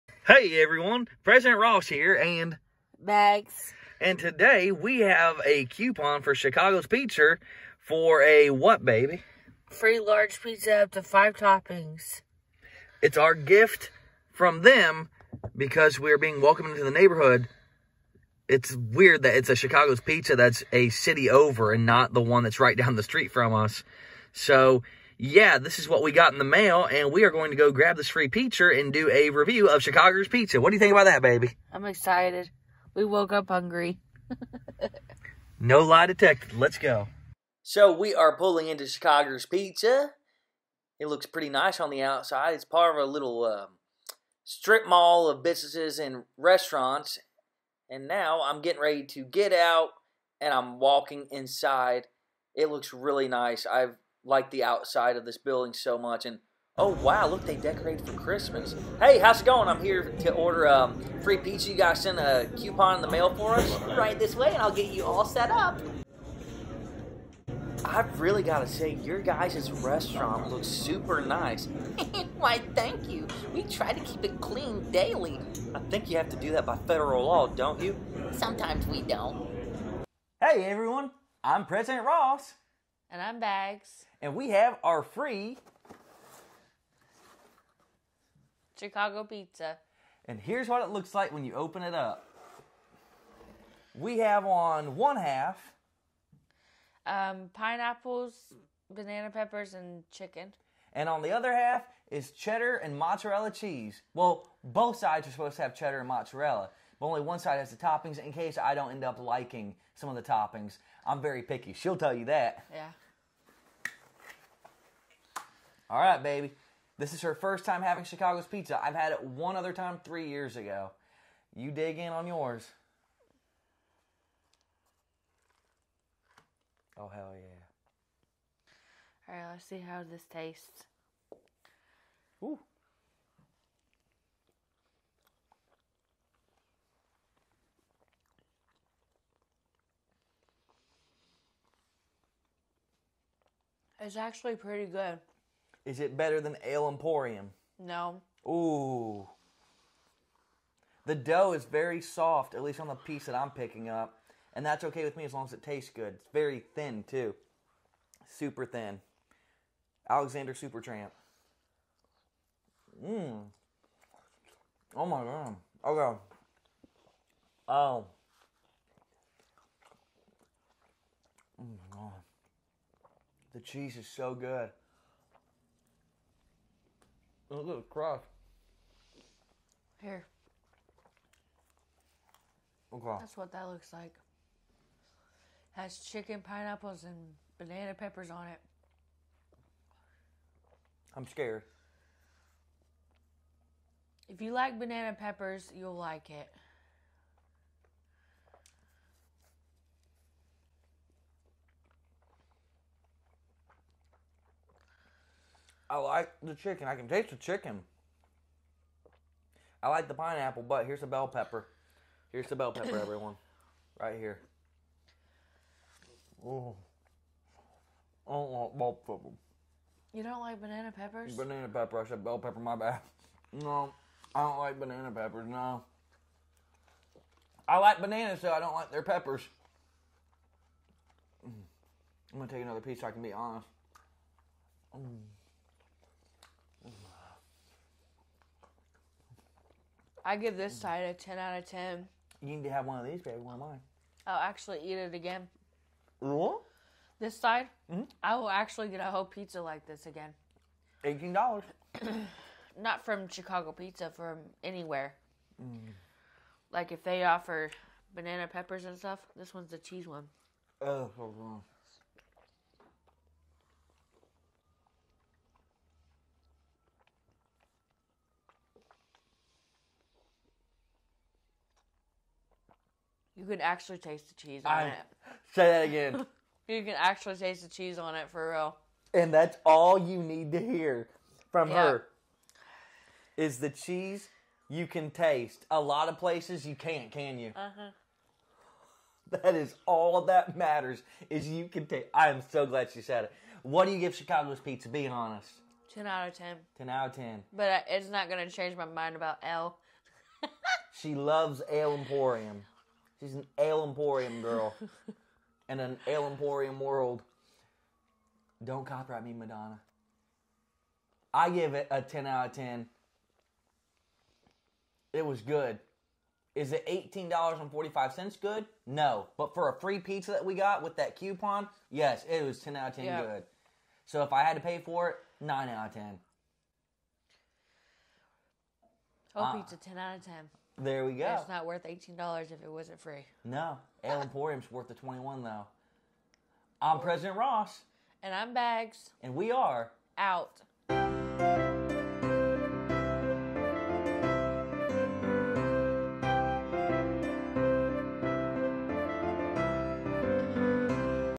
FREE Chicago's Pizza 5 Toppings Review and Taste Test
Today we eat a 5 topping FREE pizza from Chicago's Pizza was it any good!?